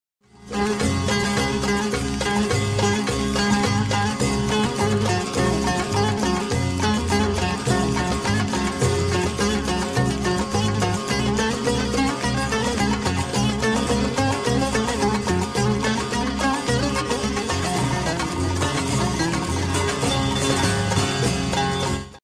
Ossi und Türke tanzen türkisch
Hier tanze ich mit meinem türkischen Mitarbeiter türkisch...